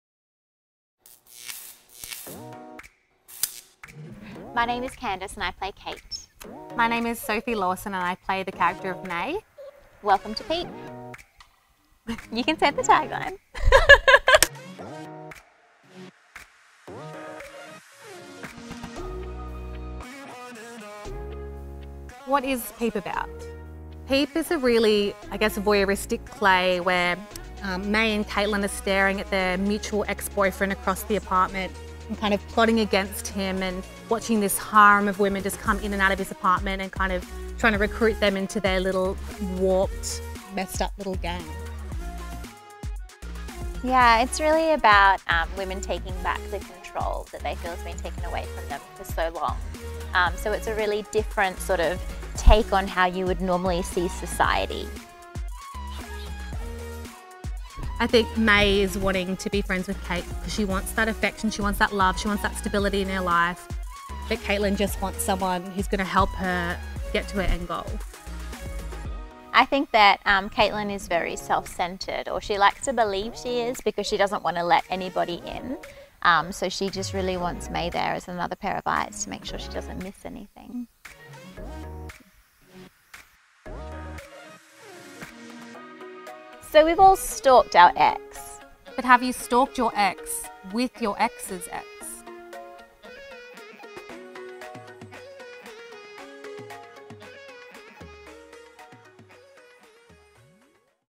Behind the Scenes video of Peep written by Jodi Gray
Behind the scenes video of Peep, written by Jodi Gray, produced and directed by The Drama Merchant, in collaboration with Reverie Theatre Company.

This show was performed in 2021 at Javeenbah Theatre, after the original location was closed down.